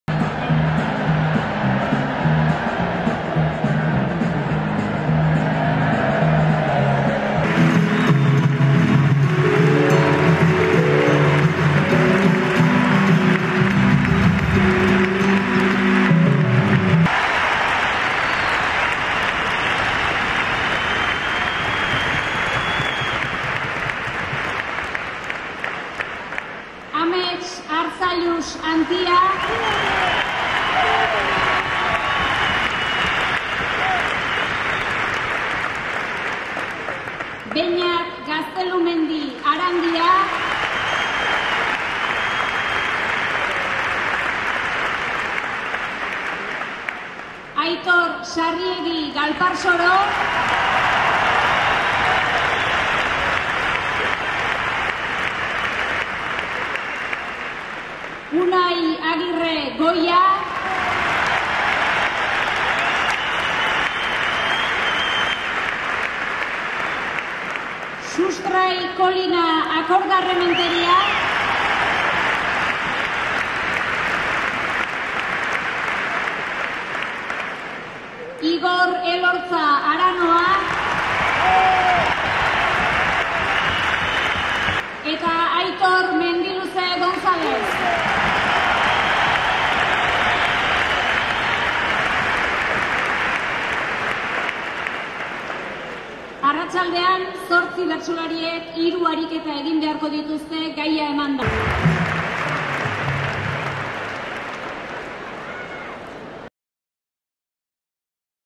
Bazkalosteko giroa BECen
| Goizeko saioa bero amaitu bada, arratsaldekoa ez da apalago hasi. Olatu artean hartu ditu publikoak bertsolariak.